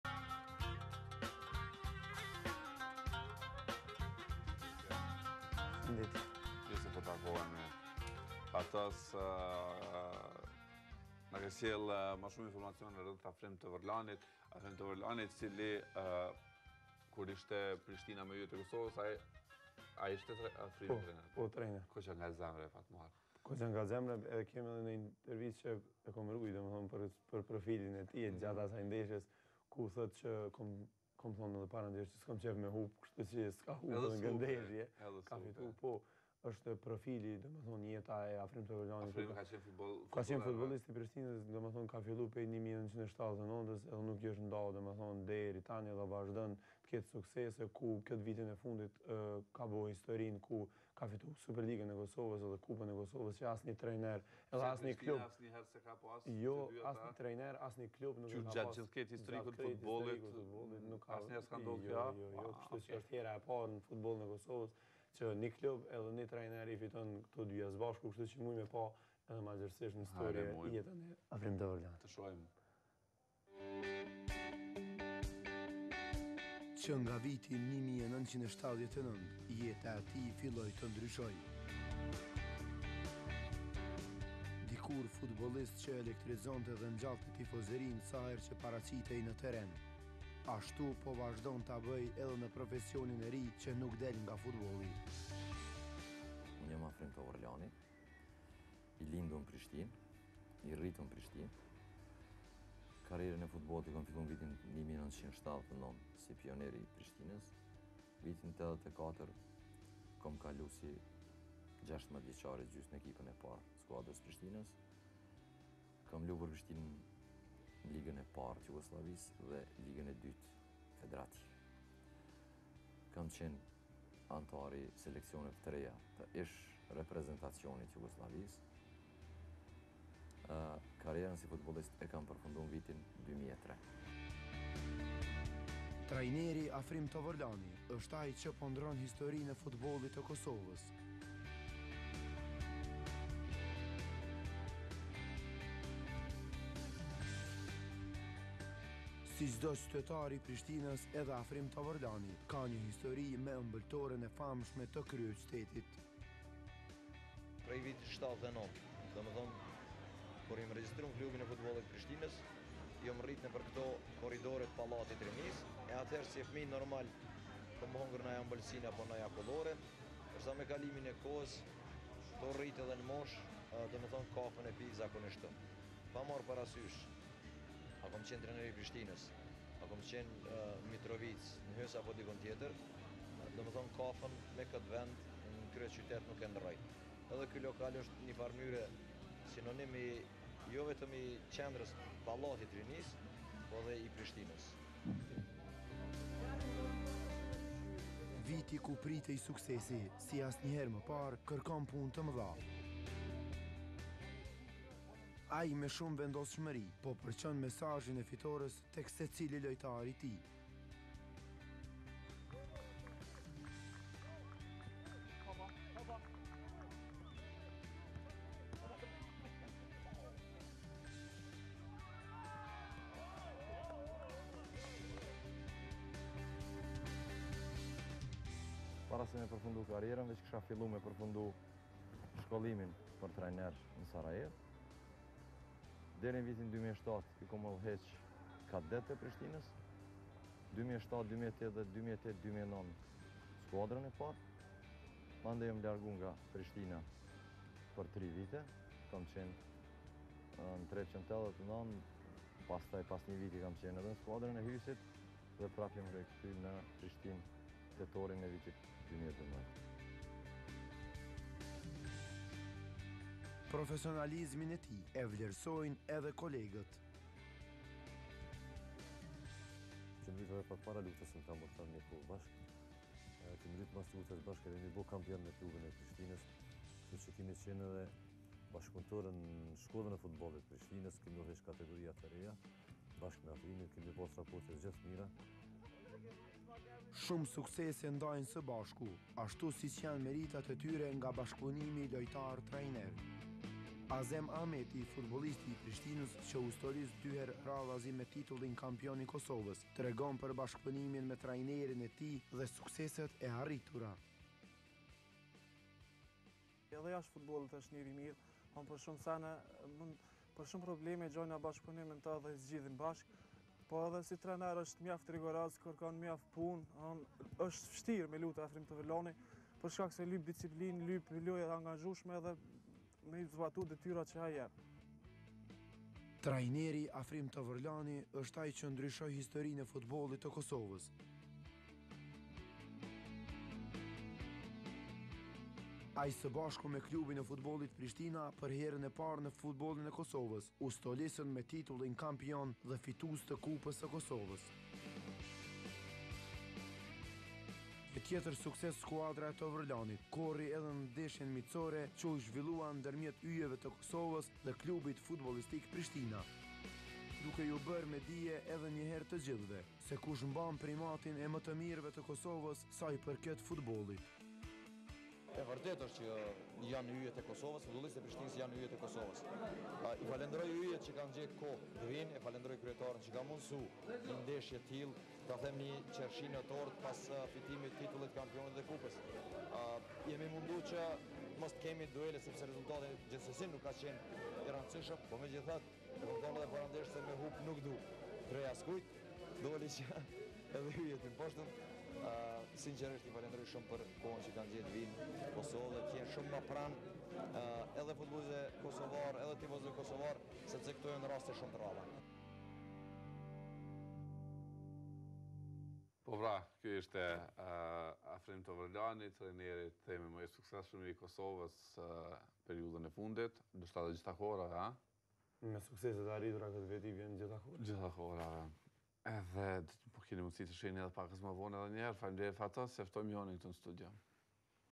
ORA 7 | Profil: Afrim Tovërlani - 01.07.2013
Ora 7 ju sjell storjen për profilin e trajnerin Afrim Tovërlani. Kur filloi të mirrej me futboll dhe cilat janë karakteristikat e karrierës së tij. Fatos Dili përmes storjes së tij sjell më shumë detaje.